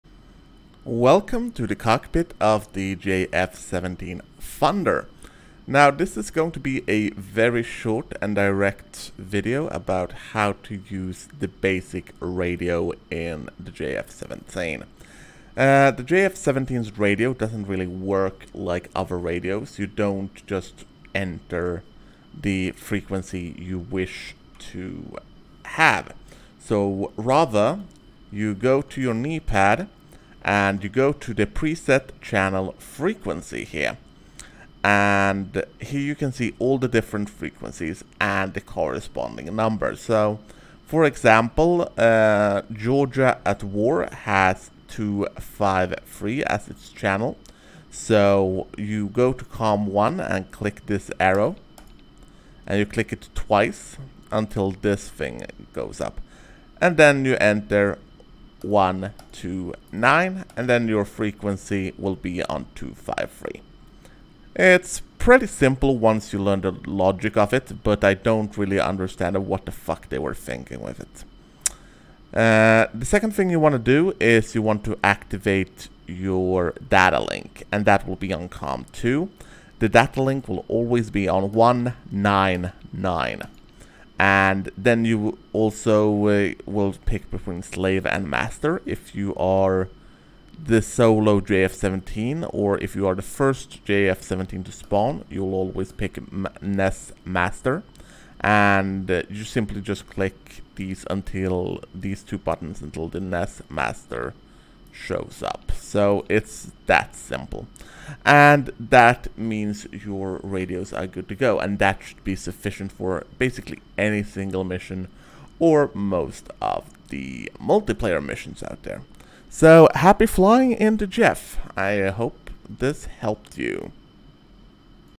A quick guide: JF-17 Thunder Basic Radio Operations
This guide is meant as a very quick demonstration of the basic operation of the rather strange primary radio of the JF-17. It is all you need to know to fly most PVE and Single Player Missions such as Operation Black Bear.